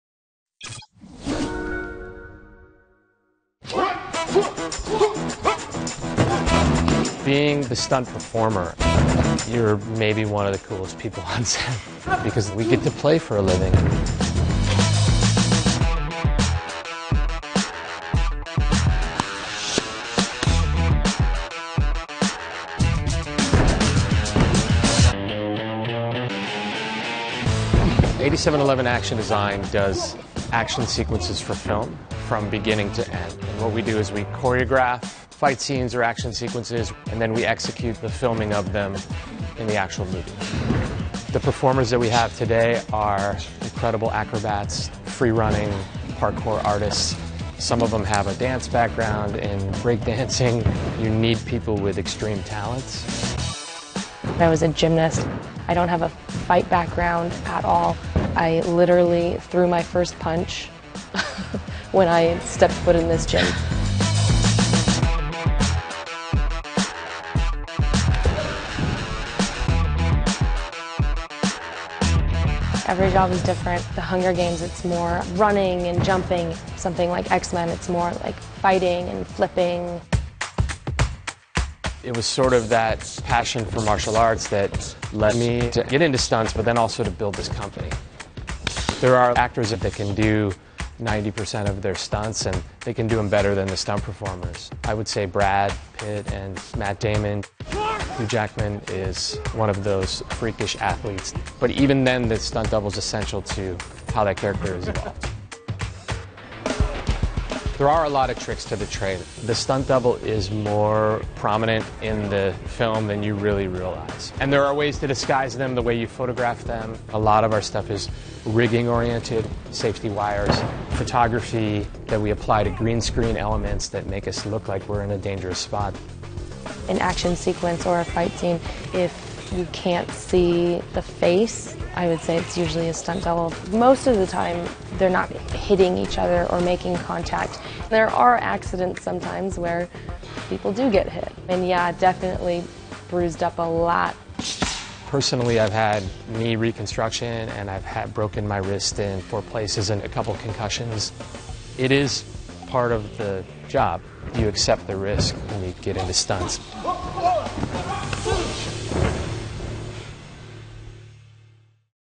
Meet Brad Pitt's Double and His Action-Movie Stunt Secrets
Aug. 11 (Bloomberg) –- Ever wonder who choreographs and designs the fight scenes and stunts for action movies? One Los Angeles-based company, 87Eleven Action Design, does just that. They are the stunt team behind more than 100 productions, including "The Hunger Games," "Wolverine," and  "Teenage Mutant Ninja Turtles." Bloomberg heads to 87Eleven's training gym to meet Brad Pitt and Jennifer Lawrence's stunt doubles and learn secrets of the trade. Video by: Jeniece Pettitt, Ryo Ikegami (Source: Bloomberg)